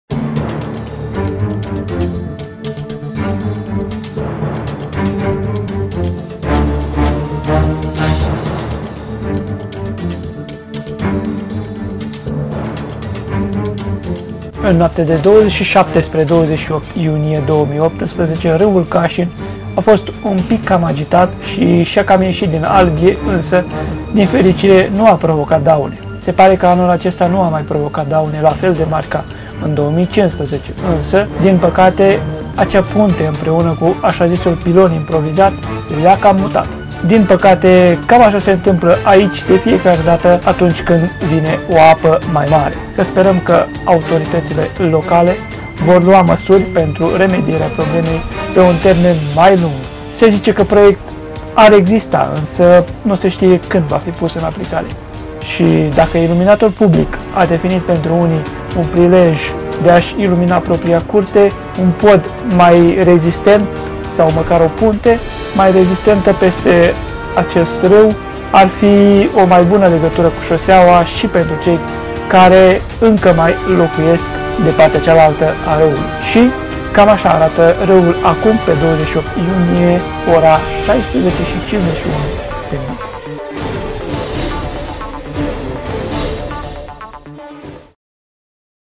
Raul Casin dupa ploaie in Parvulesti Halos | ciprian.zota vlog
MCL-News: In noaptea de 27 spre 28 iunie 2018 raul casin a fost un pic cam agitat si si-a cam iesit din albie insa din fericire nu a provocat daune.
* Pareri, intrebari. comentarii, propuneri in sectiunea de mai jos